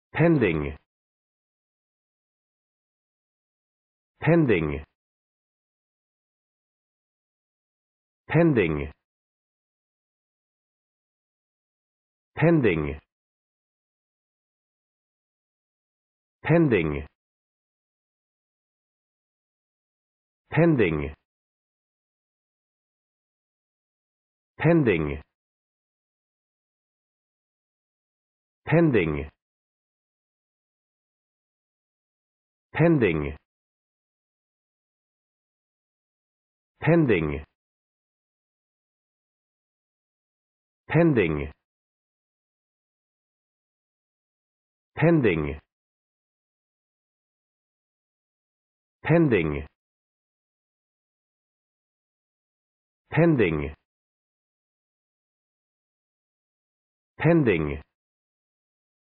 Pronunciation of Pending | Definition of Pending
Pending pronunciation | How to pronounce Pending in English?
/`pendɪŋ/
Meaning of Pending | What is Pending?
(adjective) awaiting conclusion or confirmation
What is Pending | Pending meaning and definition?